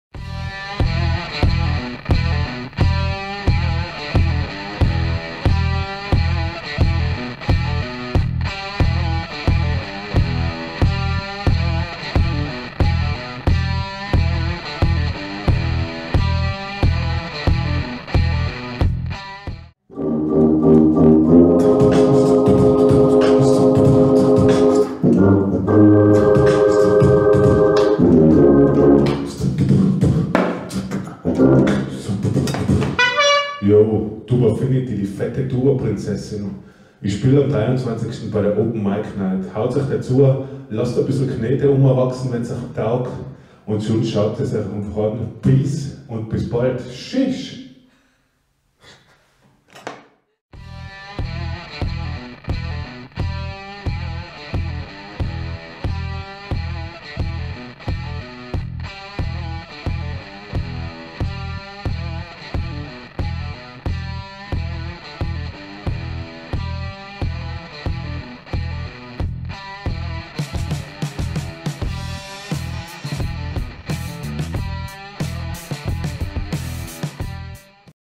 Bezirksmuseum 15: TubAffinity (open mic night April 2021)
TubAffinity lädt zur open mic night am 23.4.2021 ein - eine ONLINE-Veranstaltung

Sie haben Fragen? Sie wollen uns ein Feedback geben?